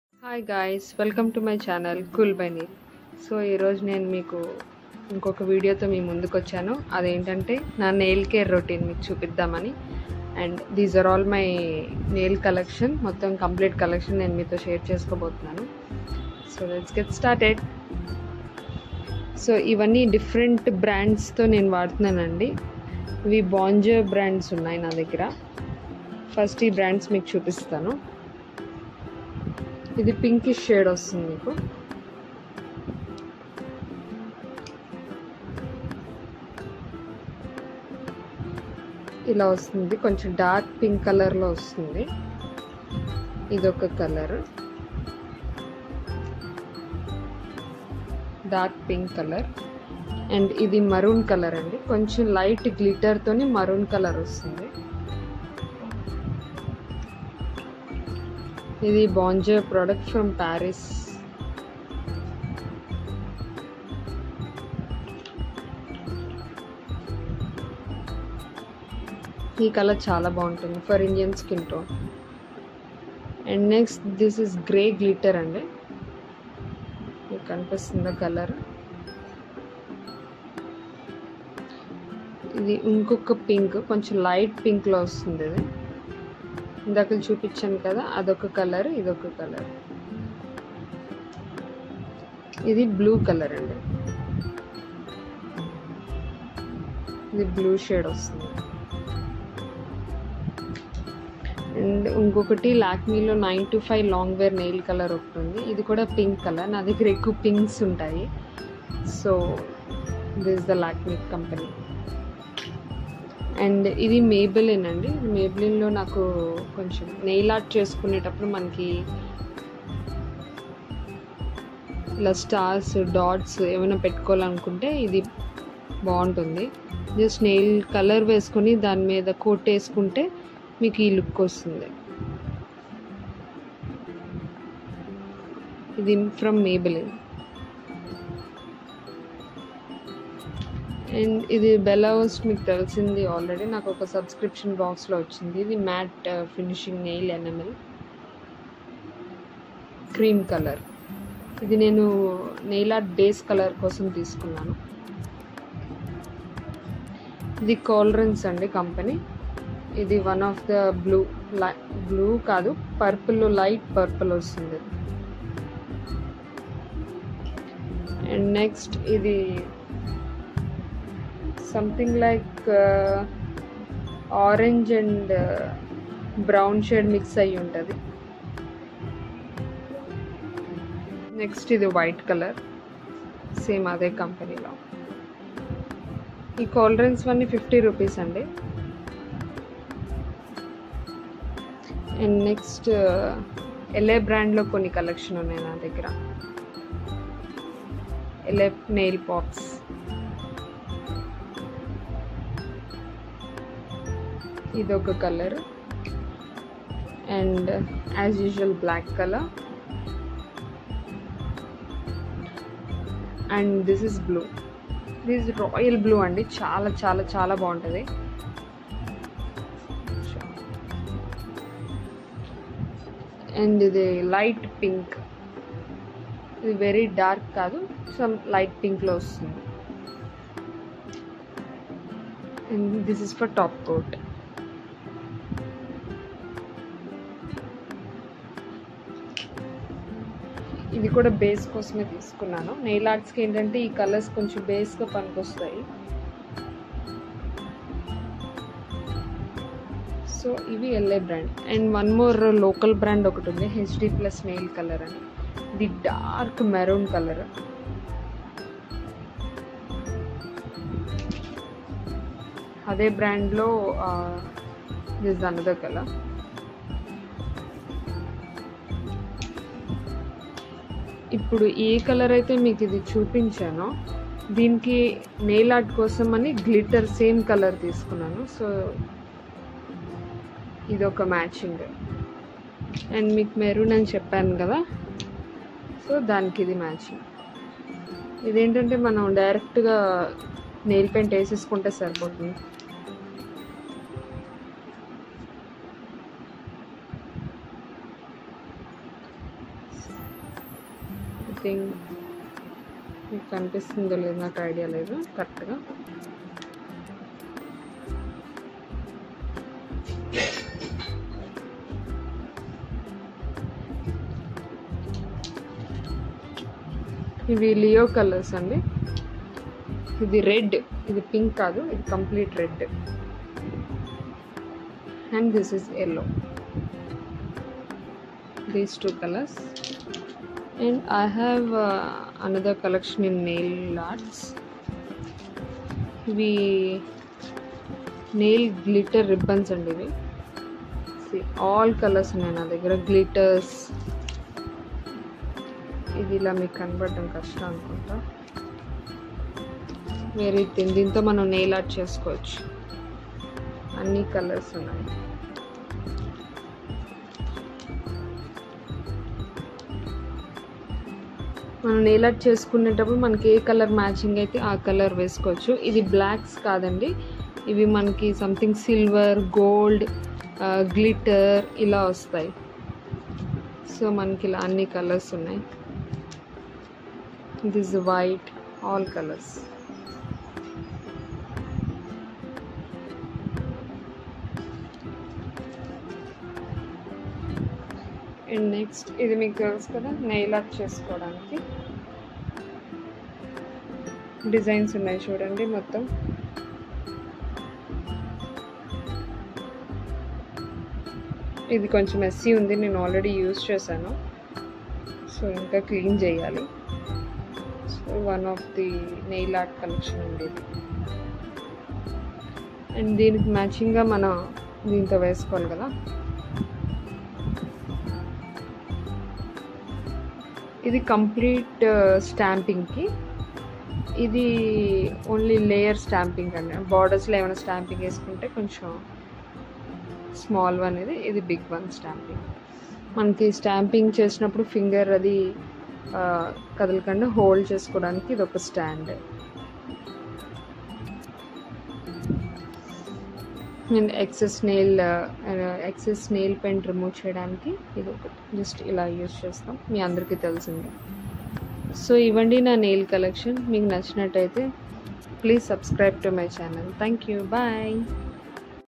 Nail art collection||nail polish collection||nail art tools||Cool by neel
Hi guys,

How r u. Hope u all good.

Today i want ti share my nail collection which is include different colors of nail polishes base nail paints, nail art tools, nail art ribbons, nail stamping and all.

Pls watch my previous videos

Cool by neel